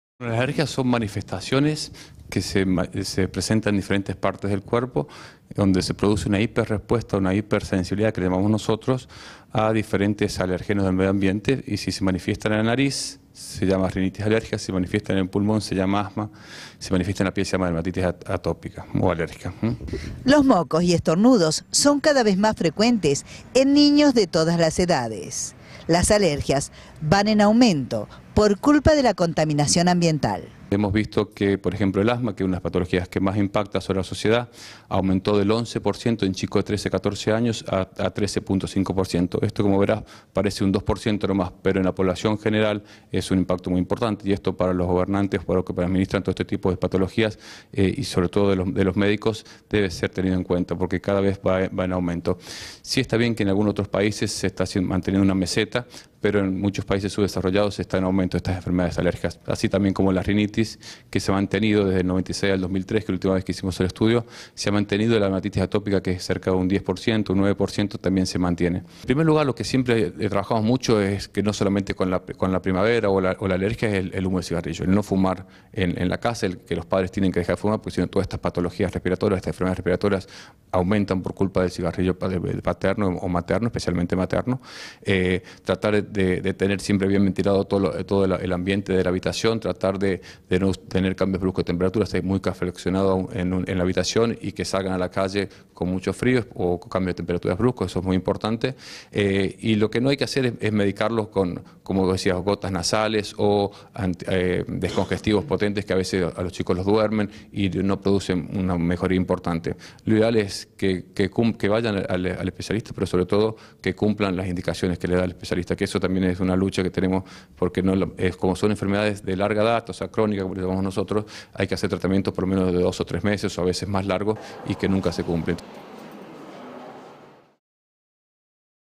Aumento de consultas por alergias en Córdoba
Se indica que en algunos grupos etáreos hay un incremento de las patologías. Recomiendan a los padres no fumar y evitar gotas.